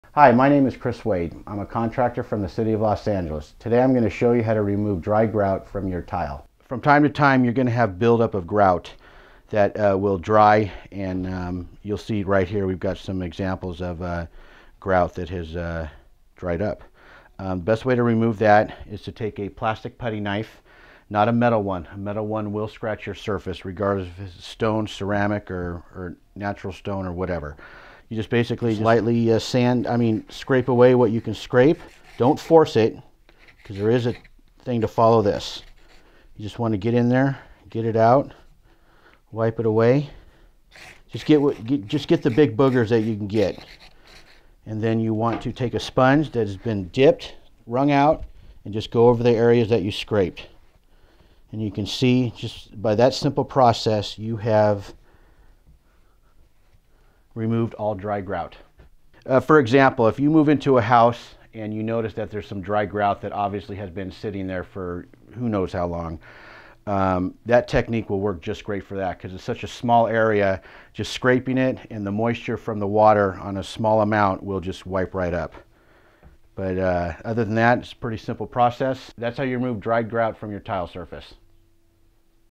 Grouting Help : How to Remove Dry Grout From Tile Surface
Dry grout is useless for defending tile surfaces, and must be removed. Get rid of bad grout with help from a professional contractor in this free video.

Expert: Chris Wade
Bio: Chris Wade has been a successful contractor for more than 23 years.

Series Description: Grouting is a process that seals away water and keeps it from damaging tile. Learn to apply grout properly with help from a professional contractor in this free video.